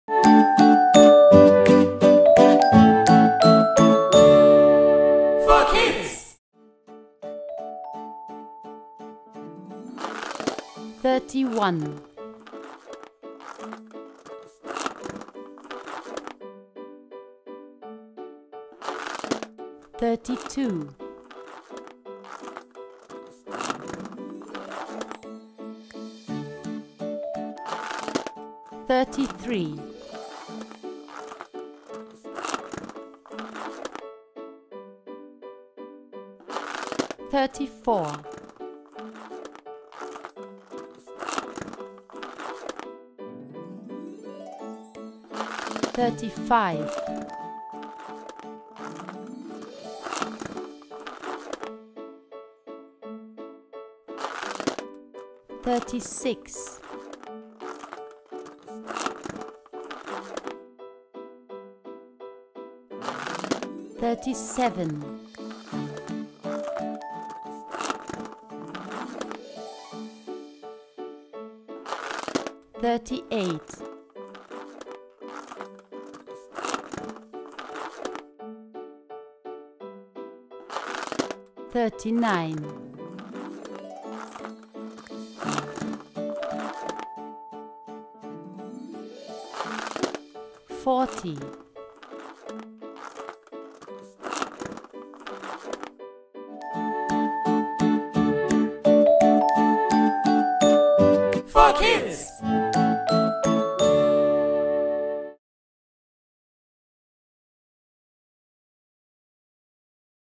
Numbers counting from 31 to 40 for children ENGLISH - Counting thirtyone to forty english
Numbers counting from 31 to 40 for children. Counting thirtyone to forty (english)
The numbers from thirtyone to forty for children to learn through audition and represented in the form of a balloon. Enjoy easy and fun with figures of 31-40 to learn counting in english.

Animal sounds, phonics and numbers for kids - learning video